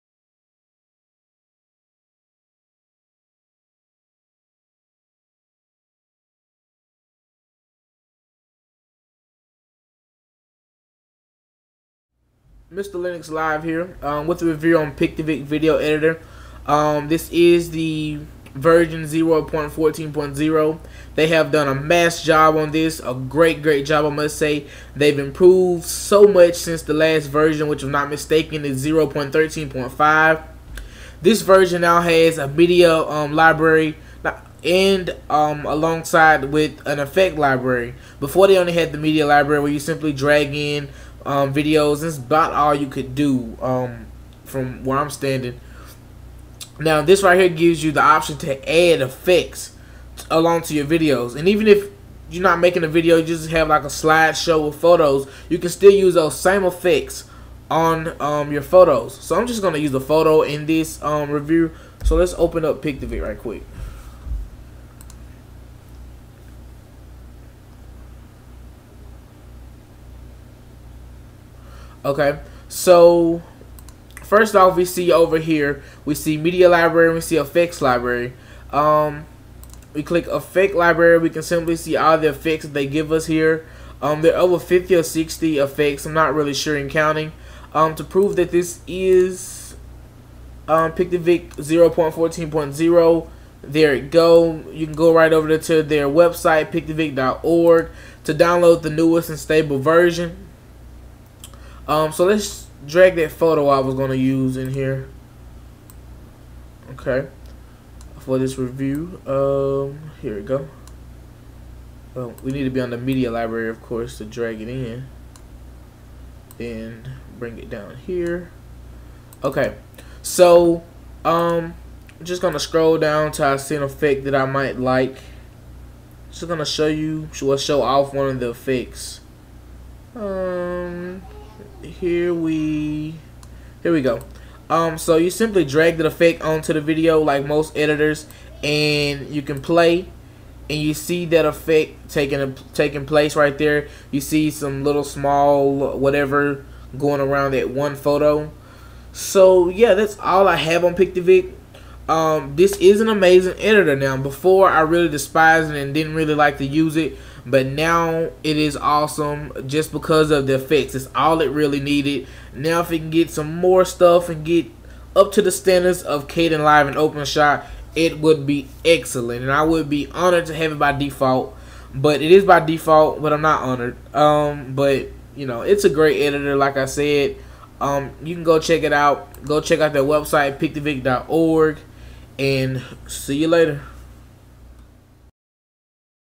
App Review: Pitivi Video Editor (v0.14.0)
What's up! This Application Review is on Pitivi Video Editor Version 0.14.0. Pitivi has changed the whole way I think of their video editing software. They have made great improvements since there last release 0.13.5.